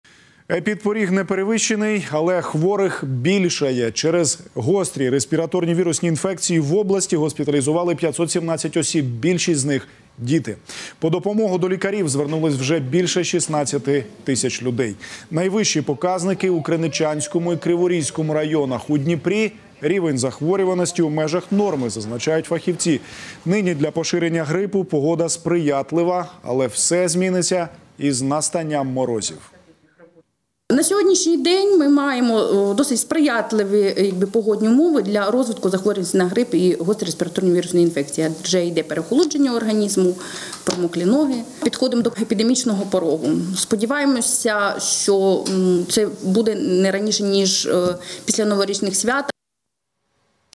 В Днепропетровской области с диагнозом ОРВИ госпитализировали 517 человек
Из-за ОРВИ в области госпитализированы 517 человек, большинство - дети. За помощью врачей обратились уже более 16 000 человек. Подробности